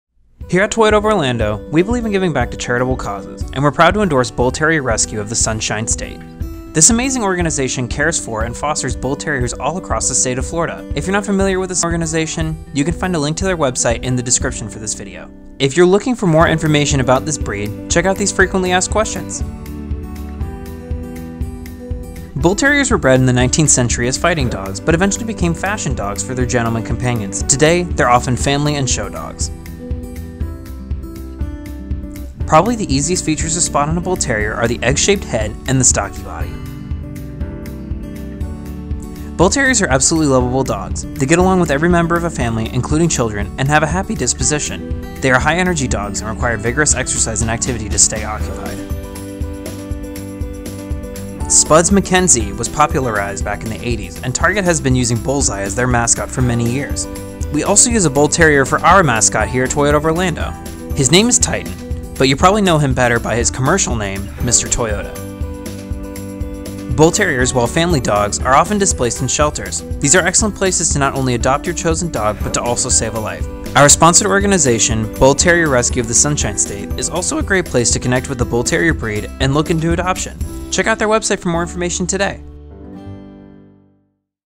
A quick guide to Bull Terriers
Have you ever wondered what it's like to own a Bull Terrier - i.e., the same breed as Mr. Toyota? Toyota of Orlando has a quick guide to these lovable pups!